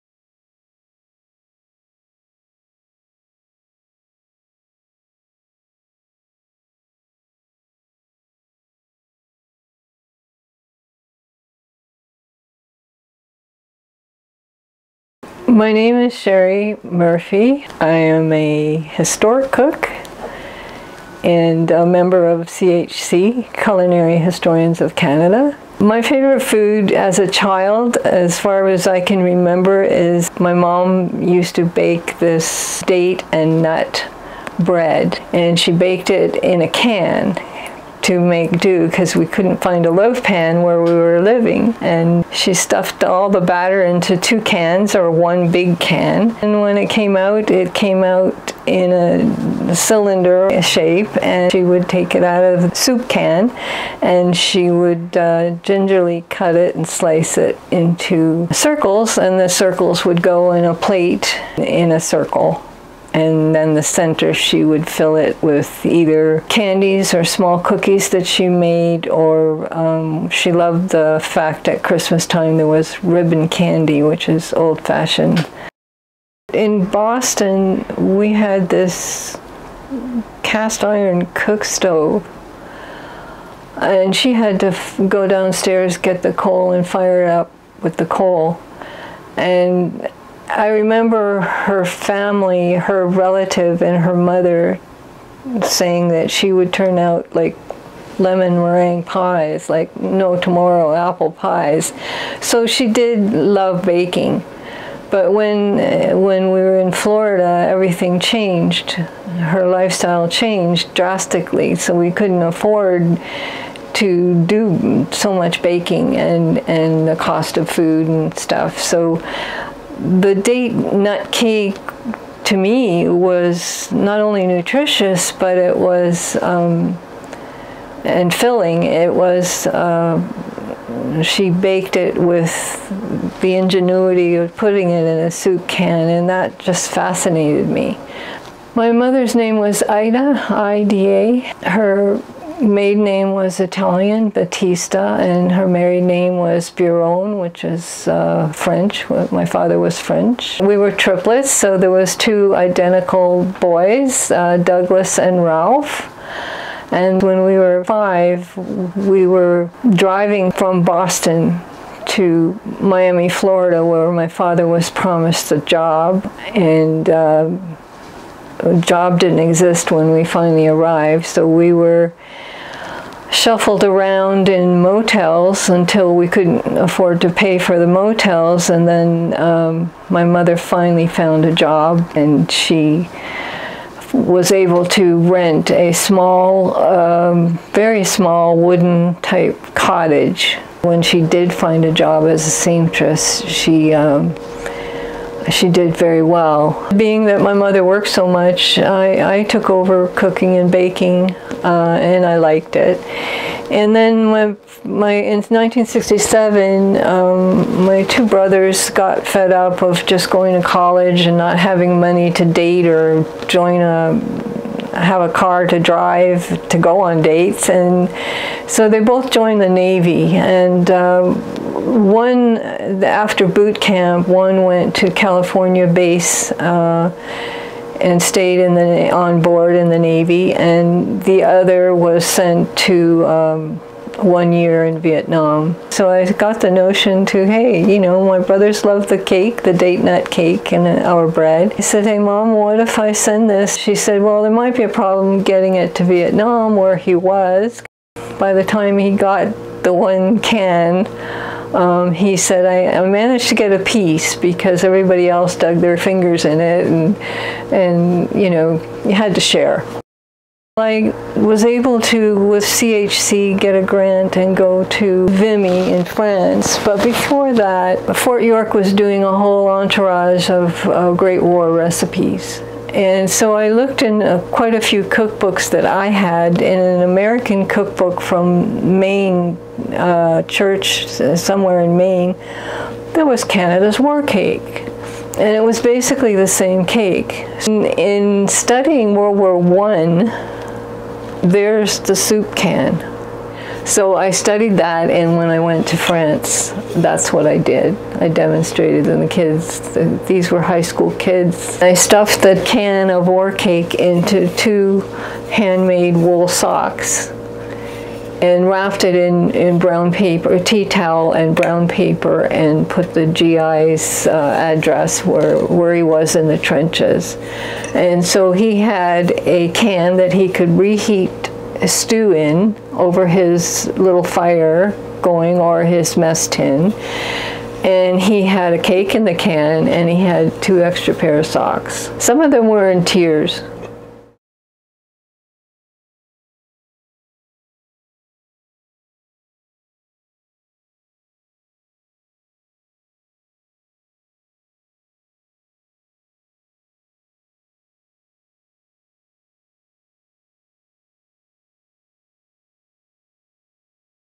Sherry's Story: Date & Nut Bread Baked in a Tin
We Are What We Ate: Sherry Murphy's mom was artistic and resourceful. Left to raise her triplets on her own, she turned a date and nut loaf baked in a soup or Chef Boyardee ravioli tin into a family favourite. Our favourite childhood food and memory project was funded in part by the Government of Canada's New Horizon's for Seniors Program. backlanestudios.ca